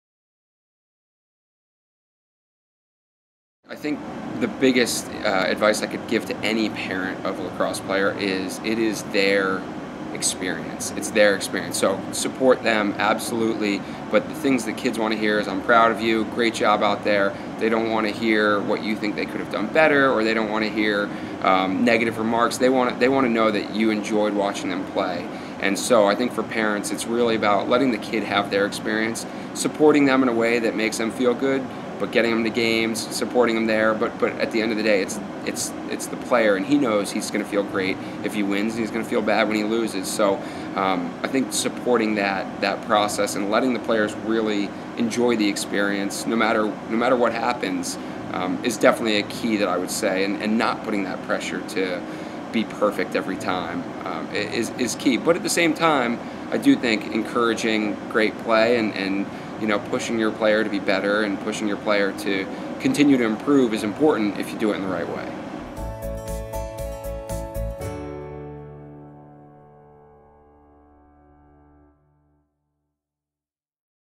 Mitch Belisle Gives Advice To Sports Parents: It's Your Child's Experience
Mitch Belisle, Professional Lacrosse Player, provides insight about parenting athletes (lacrosse players).